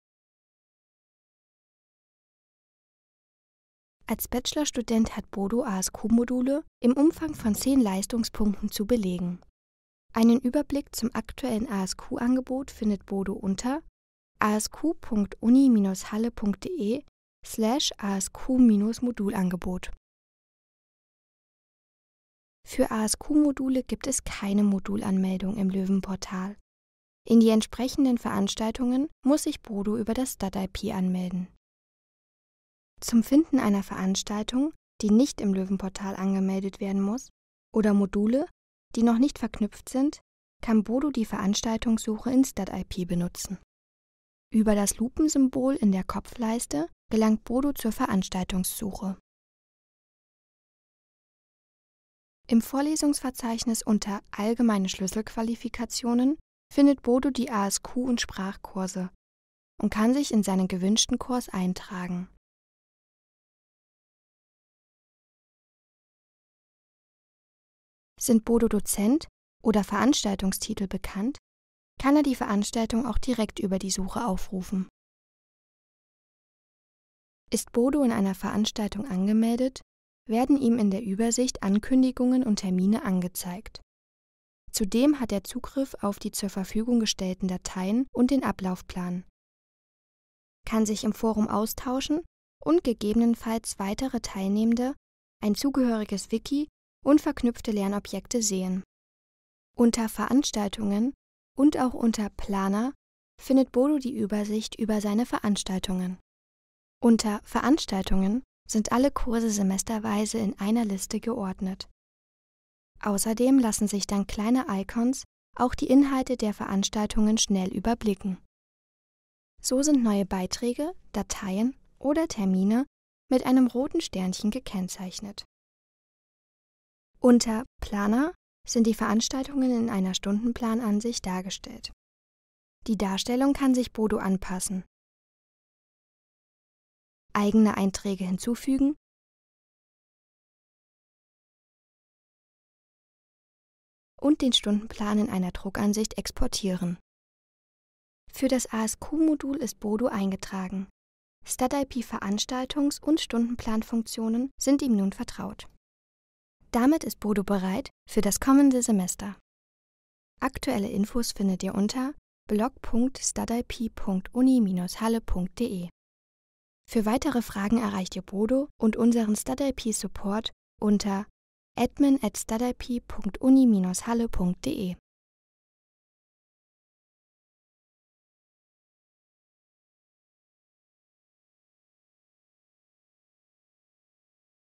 Veranstaltungen & Stundenplan im Stud.IP | Uni Halle
In diesem Video zeigen wir dir, wie du dich in Veranstaltungen im Stud.IP einträgst und deinen Stundenplan erstellst.

Weitere Informationen zu den Themen Einführung in das Stud.IP und Nachrichten erhältst du in folgenden Videos:

Video: Uni Halle